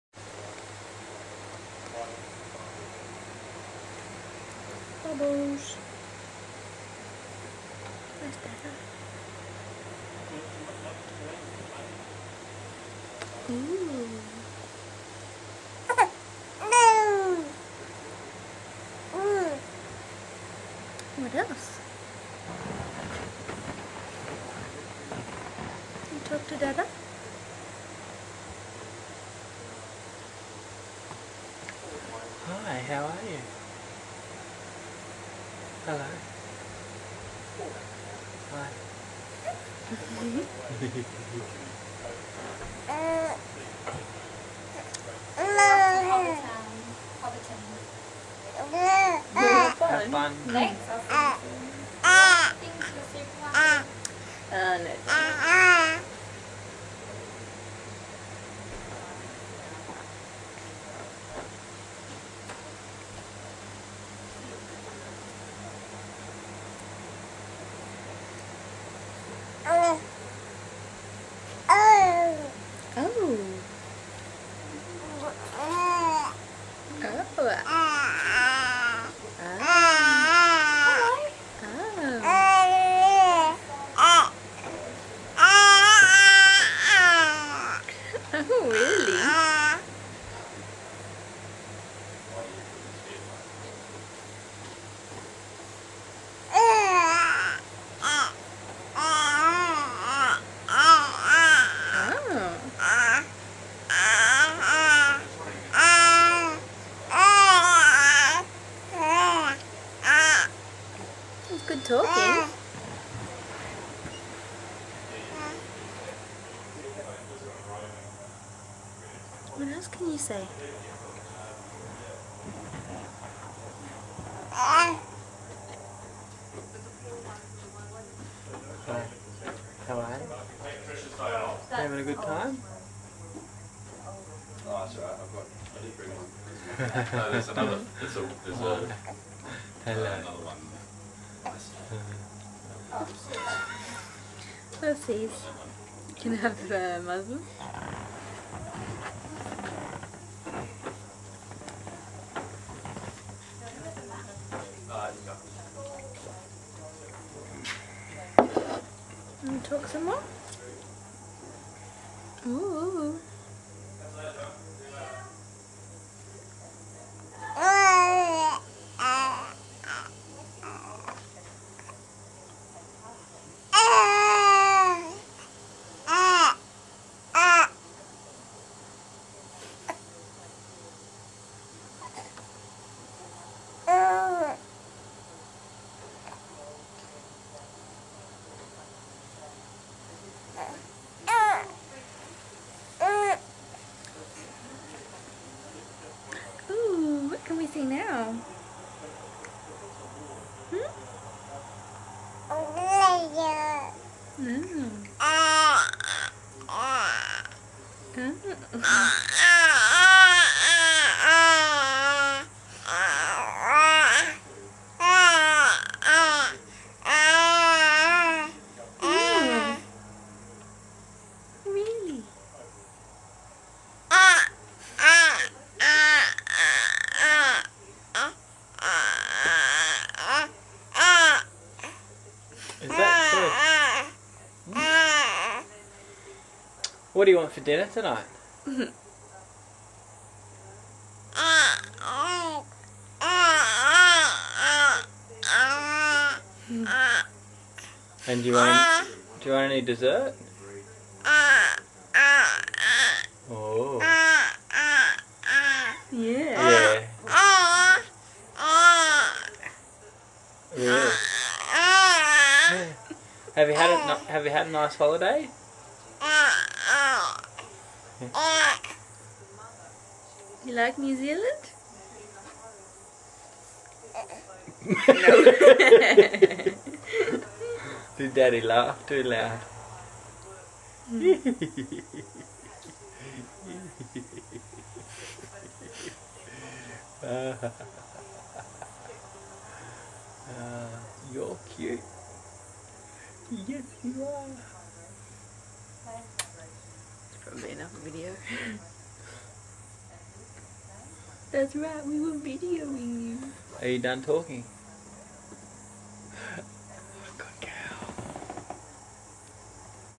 Maple Nelson having a chat with mum and dad
baby having her first conversation with mum and dad